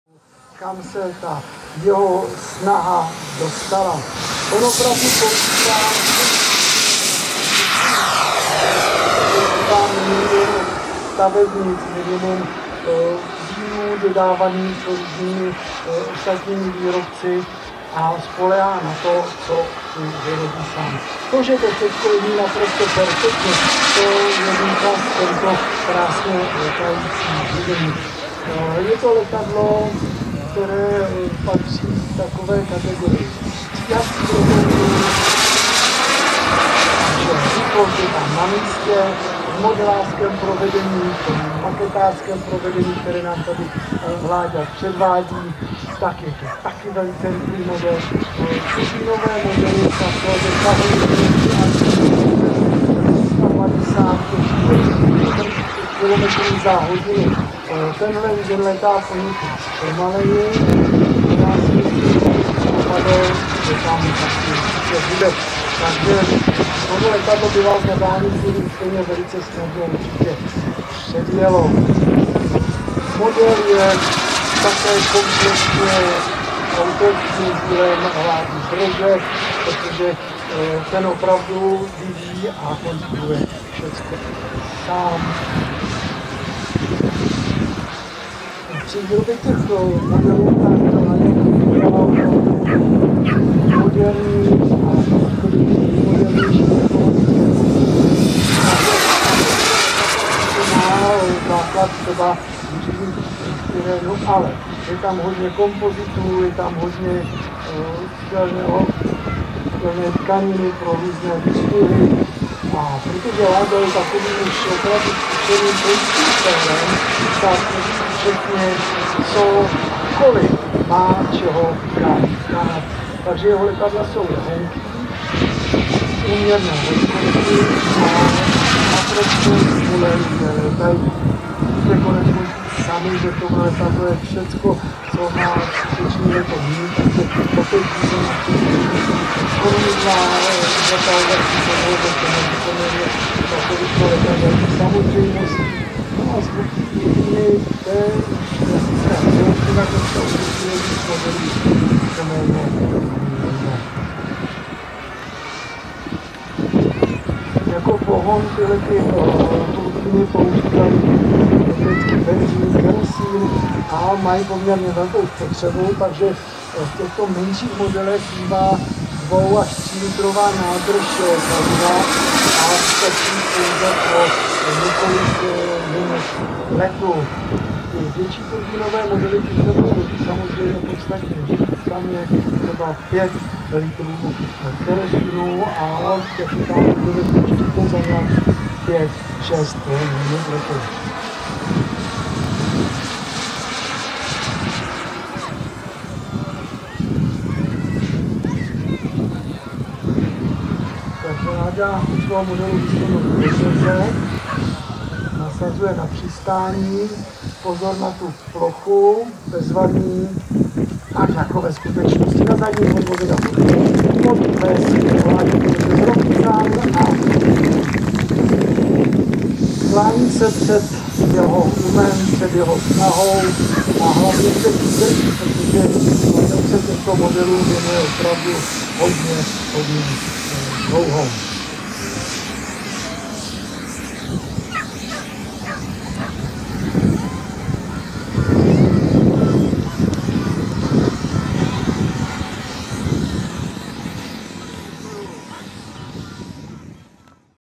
RC Saab 37 Viggen - 20.RC Model Show, Bolešiny 2019
A nicely flying scale model of the Saab 37 Viggen, a Swedish single-seat, single-engine, short-medium range combat aircraft, manufactured between 1970 and 1990. Both the model and its jet engine have been scratchbuilt by Vladislav Brož, the pilot.
20th RC Model Show, Bolešiny

Scale / Maßstab: 1:9,5
Wingspan / Spannweite: 45" (1,15m)
Lenght / Länge:67" (1,70m)
AUW / Abfluggewicht: 17,6 lbs (8,50kgs)
Engine: scratch-built, thrust: 20lbs (9kgs)